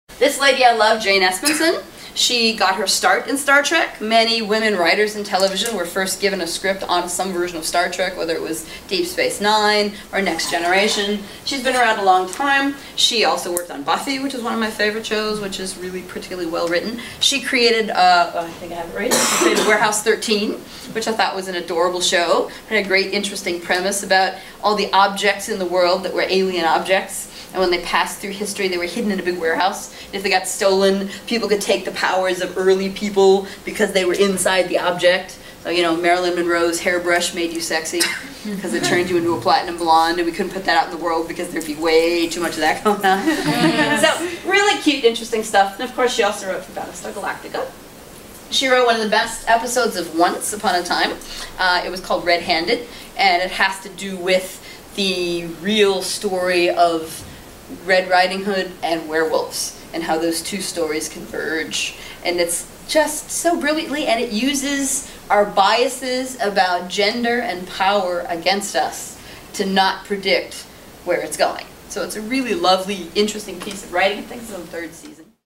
29 Jane Espenson from The Sisterhood of Science Fiction - Dr. Rosanne Welch
Transcript:

This lady I love. Jane Espenson. She got her start in Star Trek. Many women writers in television were first given a script on some version of Star Trek whether it was Deep Space 9 or The Next Generation. She's been around a long time. She also worked on Buffy which is one of my favorite shows which is really particularly well-written. She created Warehouse 13 which I thought was an adorable show and a great interesting premise about all the objects in the world that were alien objects and when they passed through history they were hidden in a big warehouse. If they got stolen, people could take the powers of early people because they were inside the object. So, you know, Marilyn Monroe's hairbrush made you sexy because it turned you into a platinum blonde and we couldn't put that out in the world because there'd be way too much of that going on. Really cute interesting stuff. Of course, she also wrote the Battlestar Galactica. She wrote one of the best episodes of Once Upon A Time. It was called Red-Handed and it has to do with the real story of Red Riding Hood and werewolves and how those two stories converge and it's just so brilliantly and it uses our biases about gender and power against us to not predict where it's going. So it's a really lovely interesting piece of writing. I think is in the third season.